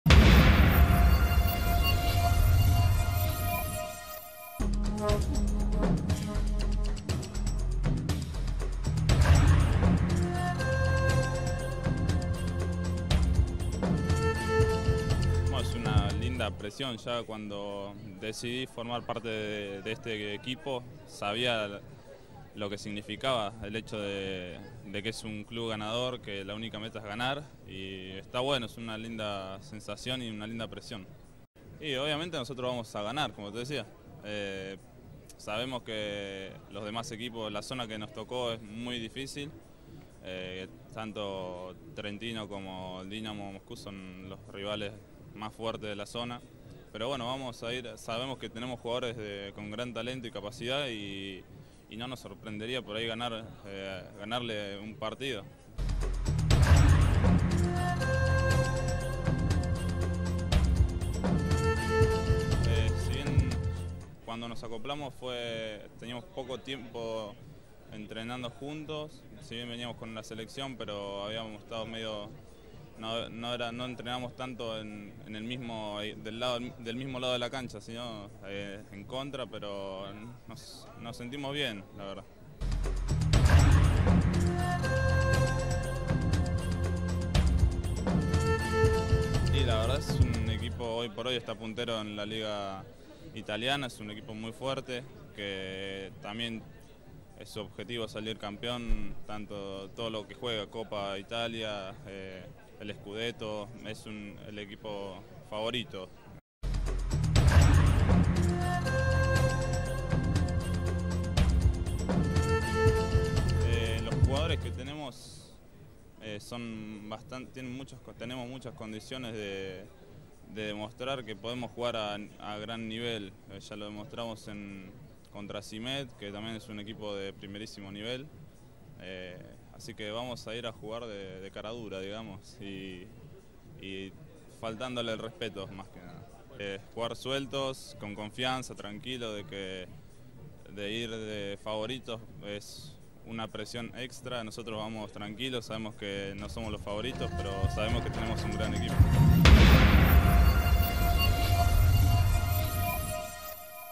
Entrevista a Lucas Ocampo Previa Mundial
Lucas Ocampo, receptor punta de Drean Bolívar, palpita el debut ante Paul Mitchell y analiza las aspiraciones del equipo argentino en el Mundial de Clubes de Voleibol